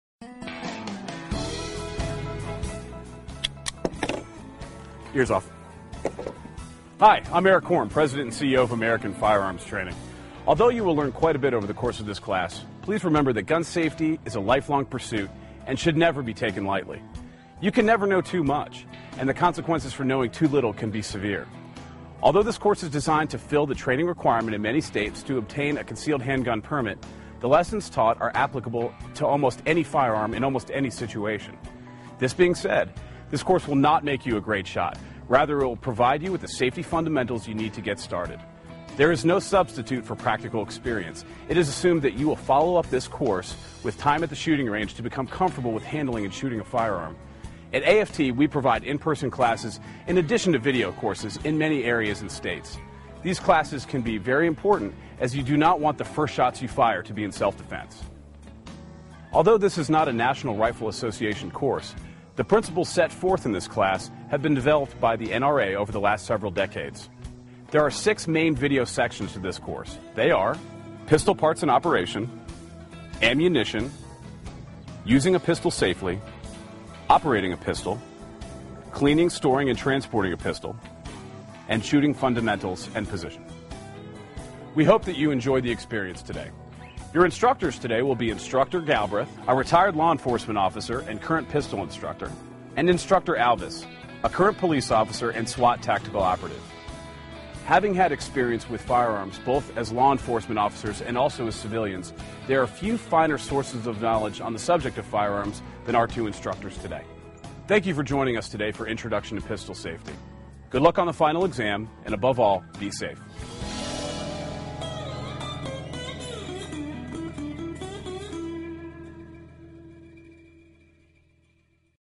Intro Video to Introduction to Pistol Safety
The introduction video to our original course, this features Eric Korn, the President and CEO of the company, discussing what our class can (and can't) do for you.  to check out the class!